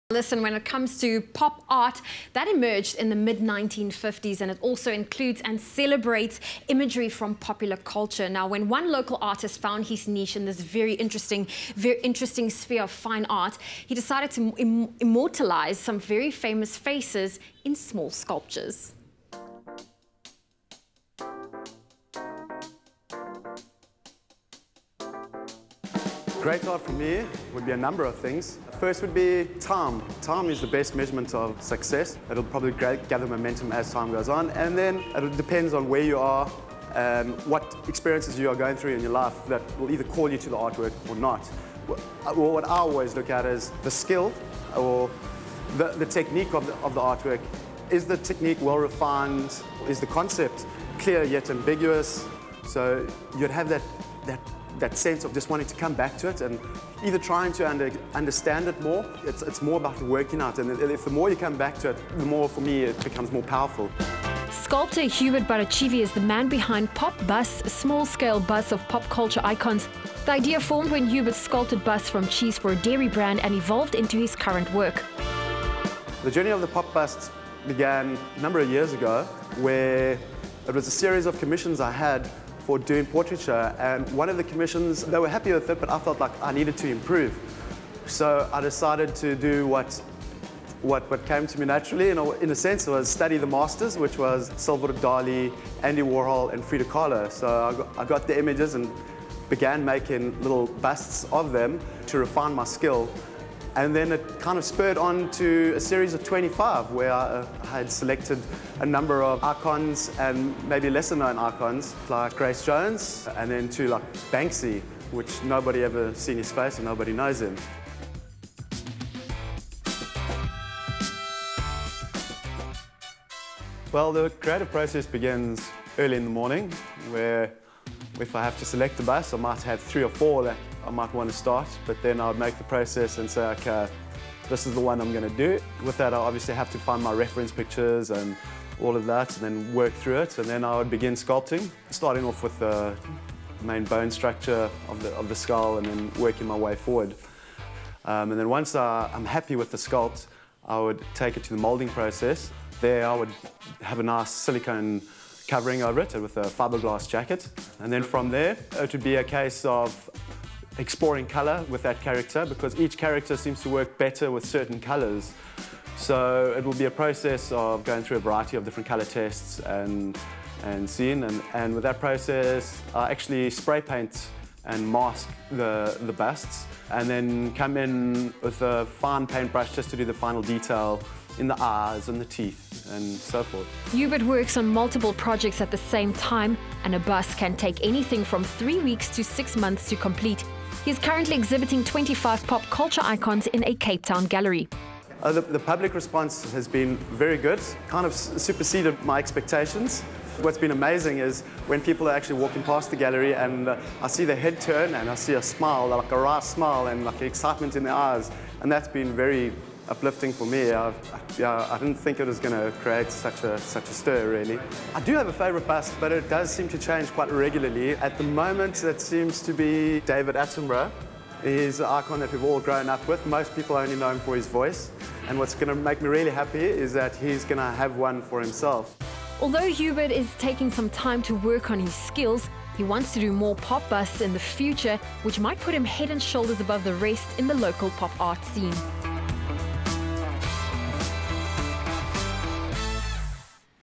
Pop Icon Sculptor, Hubert Barichievy | 22 May 2017
Pop art emerged in the mid-1950 and includes and celebrates imagery from popular culture. A local artist has found his niche in this interesting sphere of fine art by immortalising some very famous faces in small sculptures.